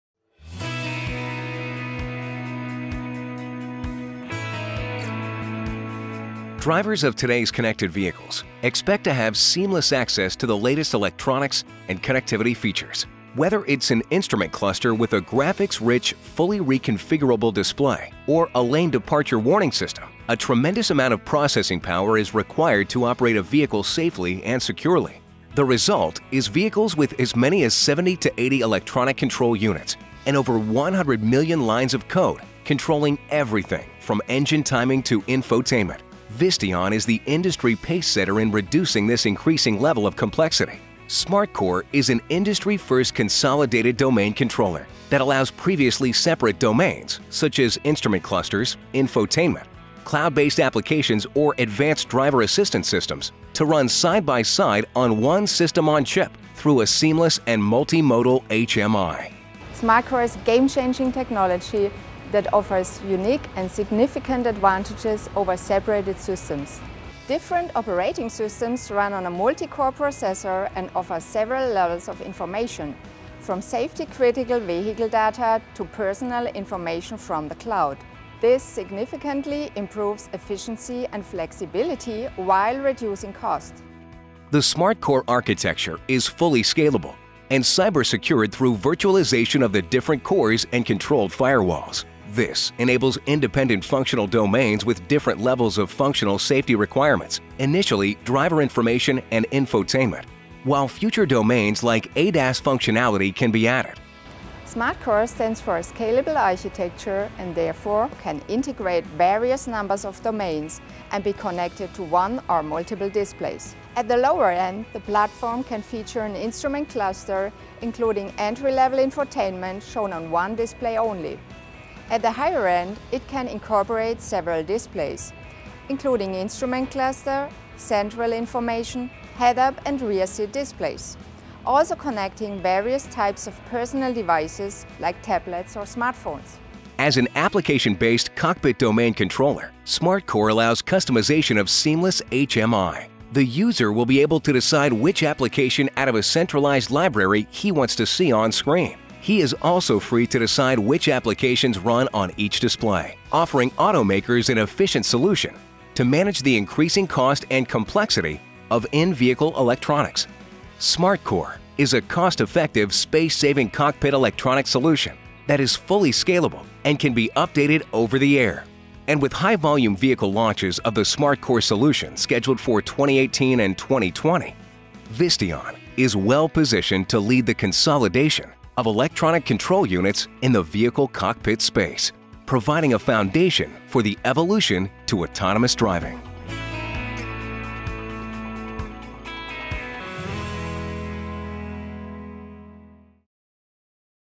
SmartCore
Consumer demand for a connected vehicle and its requirement for more electronic control units is reaching a cost and complexity tipping point. Visteon’s SmartCore™ solution provides module consolidation while bringing efficiency, safety and easy interaction to next-generation cockpits.  To learn more about how Visteon is transforming the future of mobility,